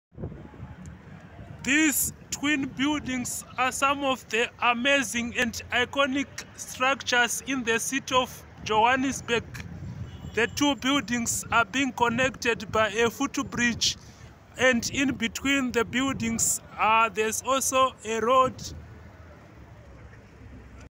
Twin Buildings, in Johannesburg CBD
The twin Buildings of Johannesburg CBD are connectedt overhead, and in between them there is a road.